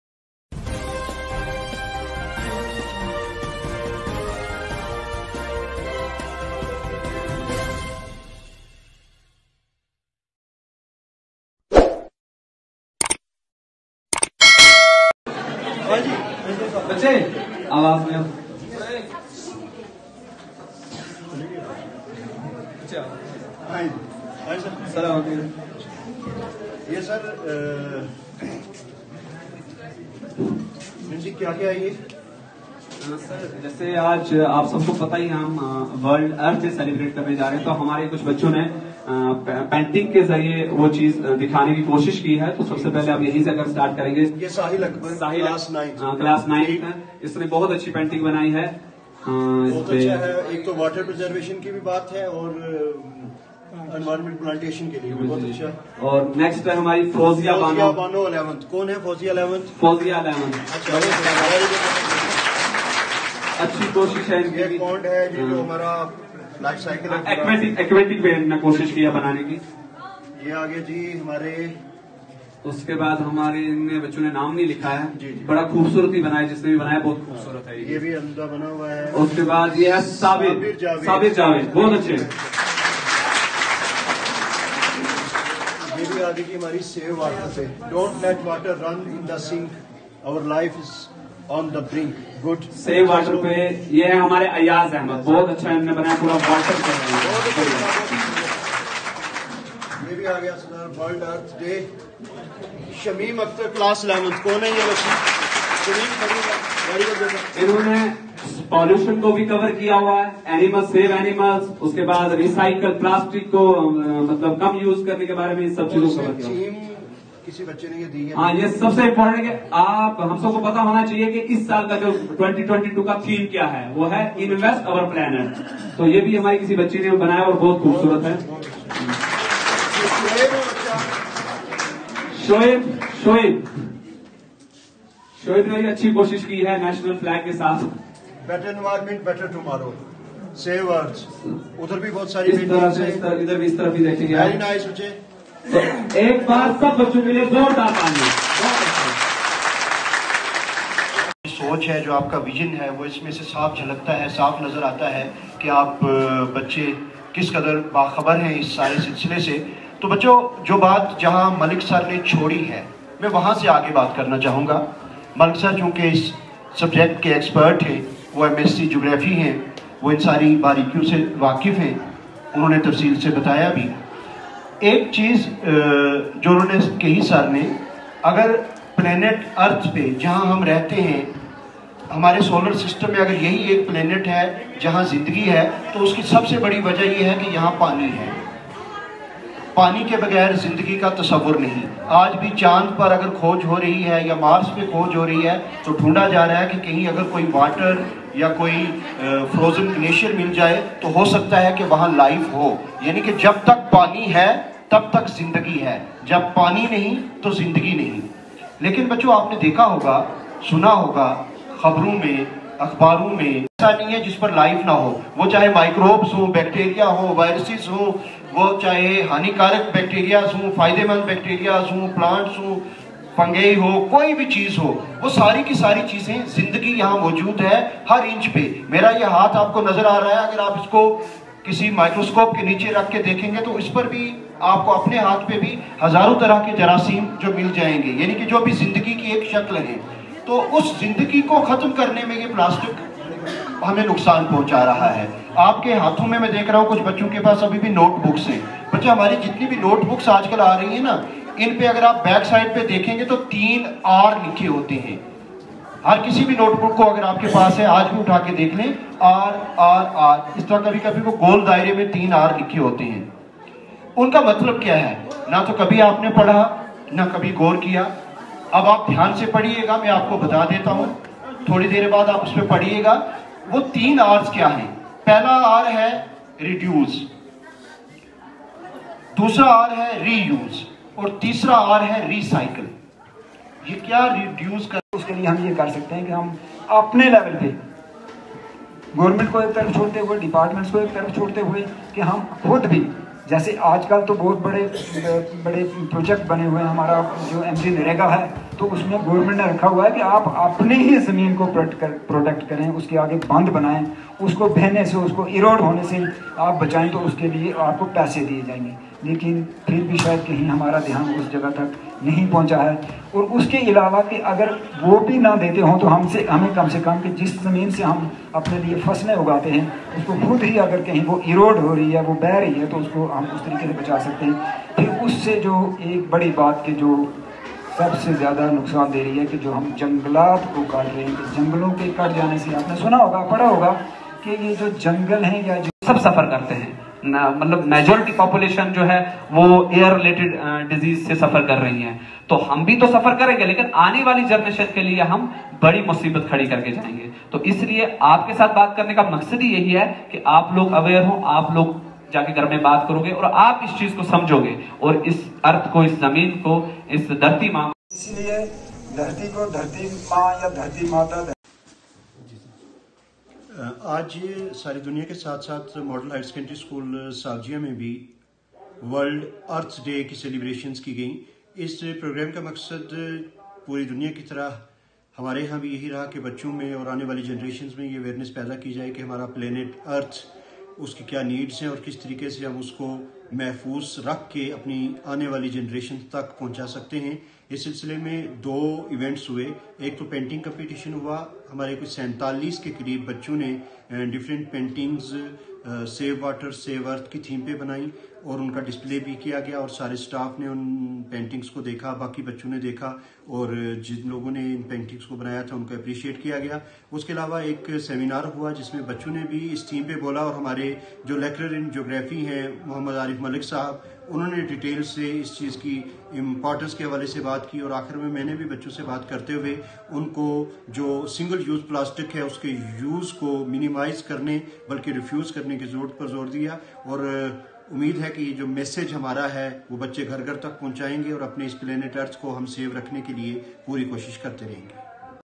A seminar and painting competition organised on World Earth Day*Poonch
*A seminar and painting competition organised on World Earth Day*
Poonch 22 April :-
Each year on April 22, the world celebrates Earth Day to create awareness about the vital need to protect our planet from pollution. In this regard  Model Higher Secondary School Sawjian Poonch organised a Seminar and Painting Competition. The students were made aware that by taking part in activities like picking up litter and planting trees, we can make our need to protect our planet from pollution.

While addressing the students,  Arif Malik Lecturer in Geography told that the day   was first celebrated on April 22, 1970, as a day of environment preservation in the US has now become a global day to promote a large-scale push for clean habitats around the world.
Moderator of the program Salim Rishi informed the audience that
this was first started when peace activist John Mc Connell proposed to honour the planet and the concept of peace during a UNESCO conference in San Francisco.
The day also reminds us how fragile our planet is and how it is important to save it from the global climate crisis that is worsening with each passing day.
While addressing the gathering Principal Model Higher Secondary School Sawjian Anwar Khan said that the theme of Earth Day 2022 is to 'Invest in our planet', urging businesses to shift towards sustainable practices. He also stressed upon the students to avoid using single use Plastic and avoid littering around. The others who spoke included Parvez Khan, Balbir Singh, Firdose Anjum, Abdul Ghani , Rashmi Sudan and Sarfraz Ahmad.